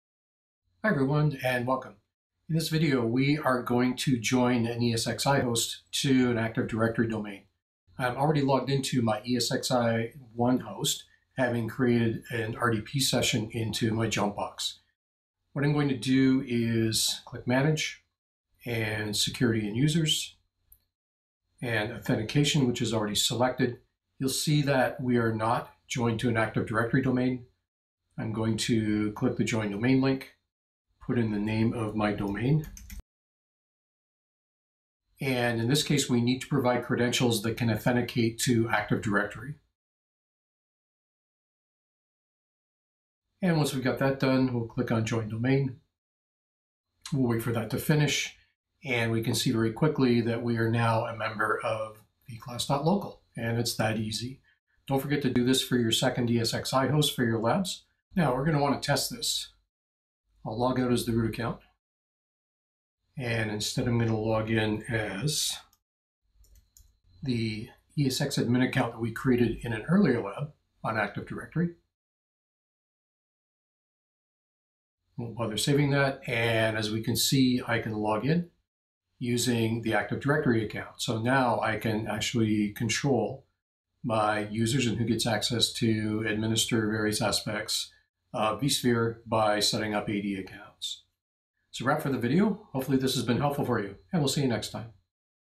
Joining ESXi to AD in vSphere 7
This video demonstrates joining ESXi to an Active Directory domain and testing logging into ESXi with an Active Directory user in vSphere 7.

This is a video in a series to demonstrate the completion of labs that build out and configure a nested vSphere 7 environment that is used in several courses in the school of ASCIT.